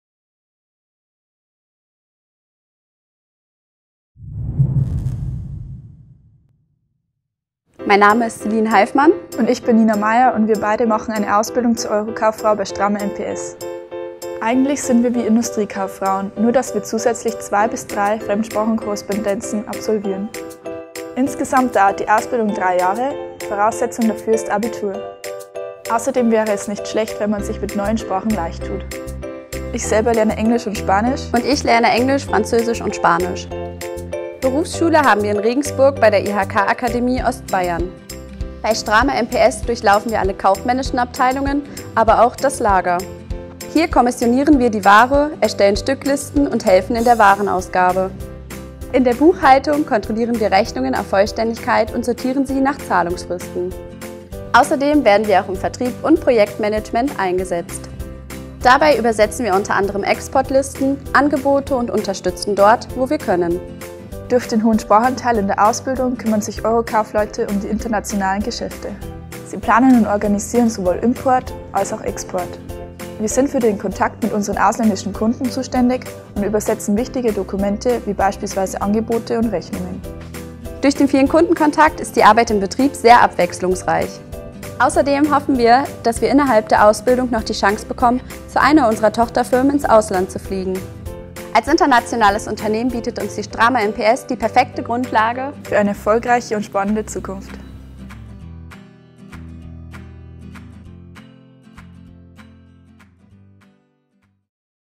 Ausbildung Eurokaufleute bei Strama-MPS (m/w/d)
Ihr interessiert euch für eine kaufmännische Ausbildung und wollt den Kontakt mit internationalen Geschäftspartnern?

Dann ist die Ausbildung zum/zur Eurokaufmann/-frau genau das Richtige für euch!

Nina und Celine zeigen euch, was sie während ihrer Ausbildungszeit alles lernen.

Wir freuen uns auf eure Bewerbungen!